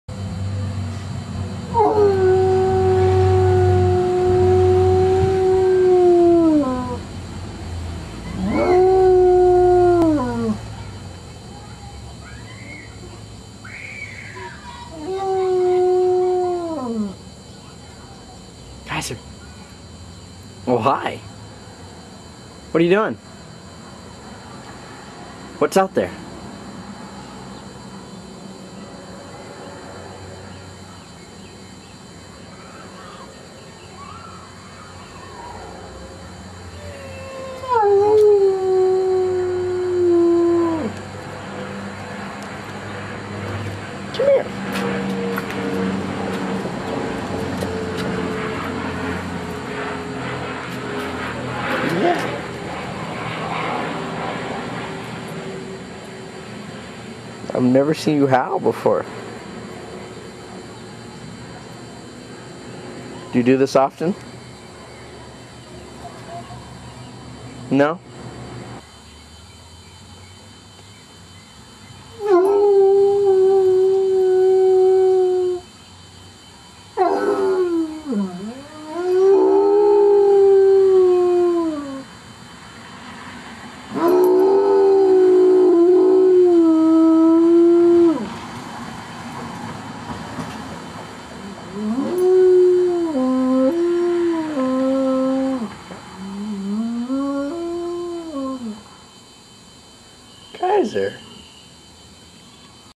Kaiser Howling
Our dog Kaiser is caught on tape howling for first time. In the two years that we have had him, he rarely even barks. It was interesting to see him howl (for no apparent reason)